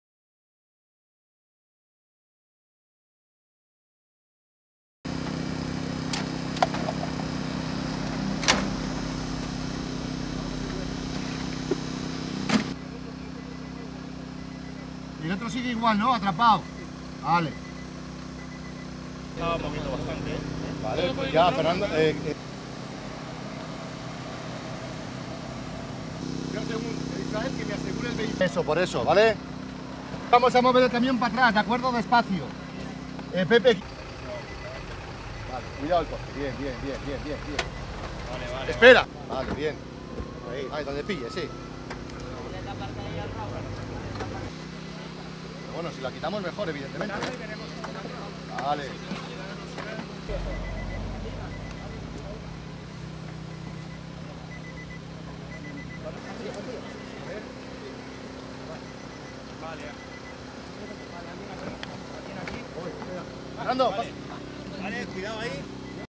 Tres heridos en un accidente en la Pobla Tornesa (Castellón)
Tres personas han resultado heridas este viernes con contusiones y traumatismos en un accidente entre una furgoneta y un coche registrado en la CV-160 en el término municipal de la Pobla Tornesa (Castellón).